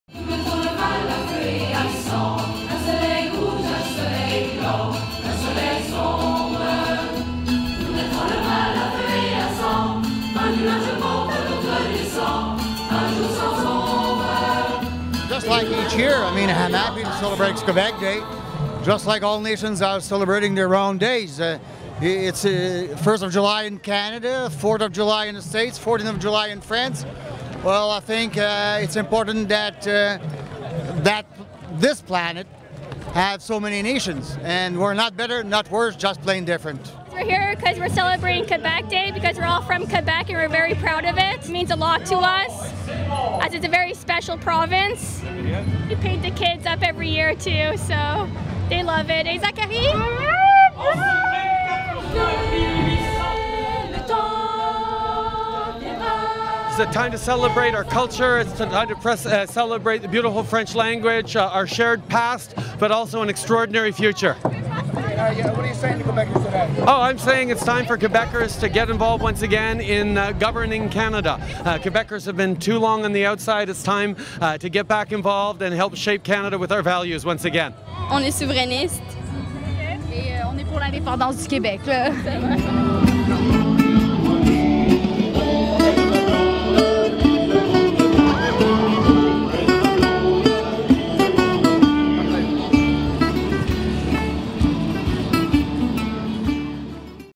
Fête nationale parade in Montreal
Quebecers, including sovereignist and federalist politicians, were out to celebrate the Fête nationale at the St. Jean Baptiste parade in Montreal Wednesday, June 24, 2015. (John Kenney / MONTREAL GAZETTE)